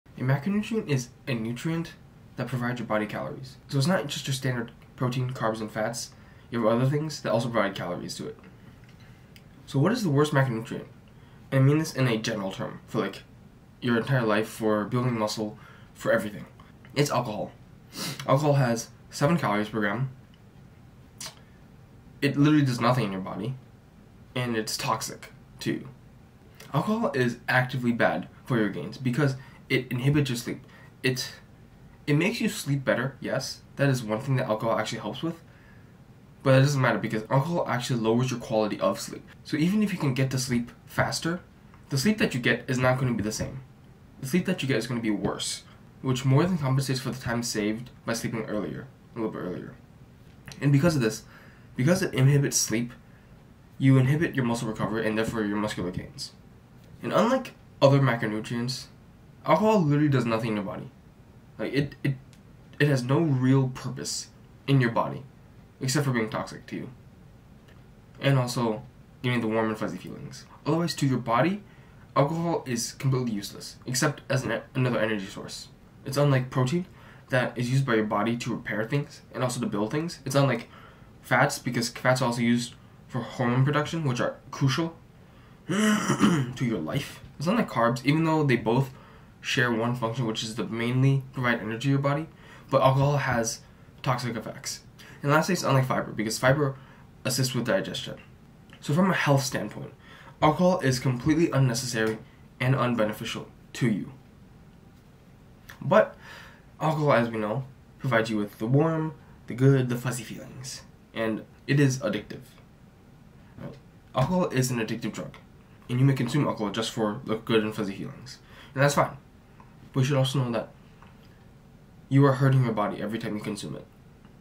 This is the WORST Macronutrient!
Out of all the macronutrients (nutrients providing calories), this is the worst out of all of them nutritionally. On the other hand, it could also be argued to be the best from another perspective.